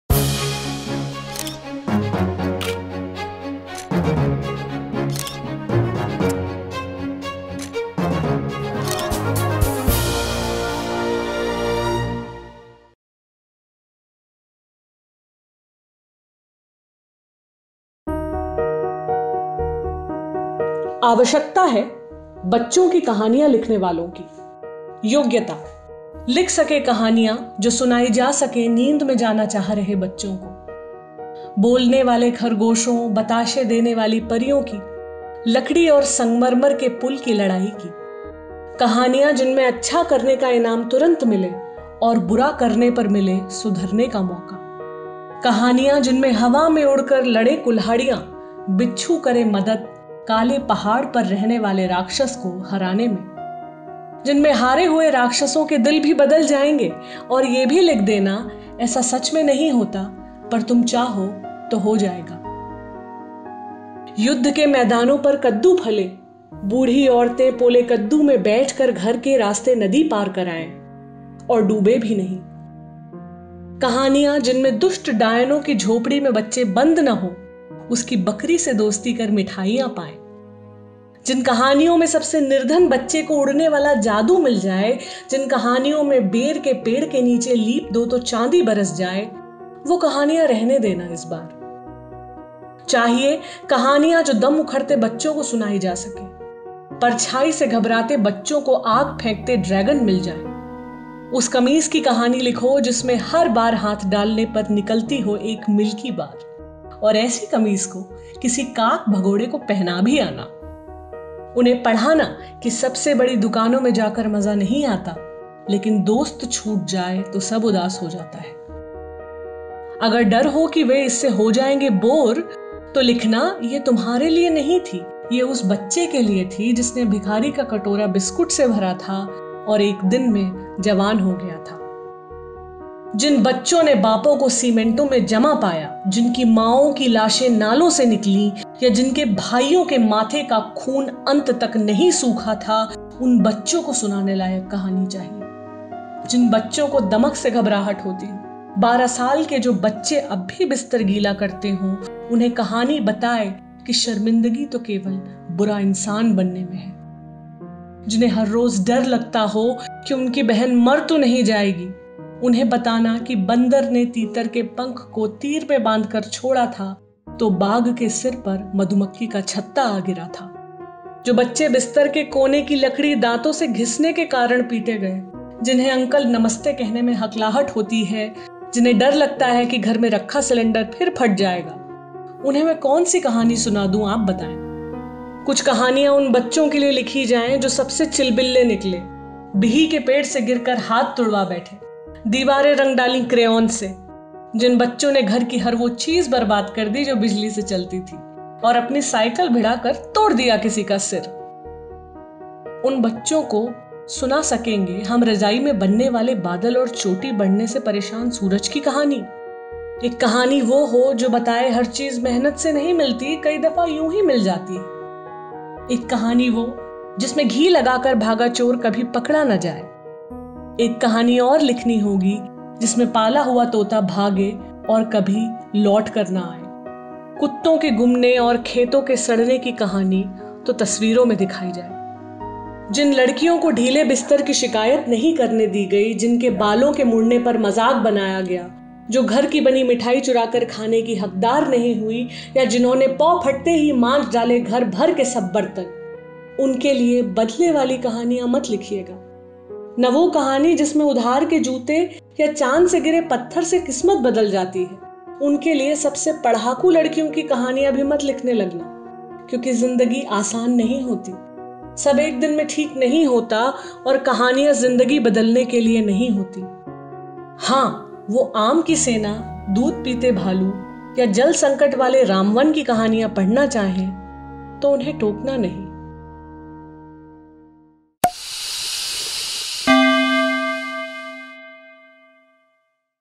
Tasveer:आवश्यकता है, बच्चों की कहानियां लिखने वालों की | A writer for children | Poem| Ashish Mishra
In this edition of our special series ‘Tasveer’ Poem 'A Writer For Children' by Ashish Mishra. Our world is seriously missing a writer for children, here is JD (Job Description), see if you are eligible.

Edited By: Amitesh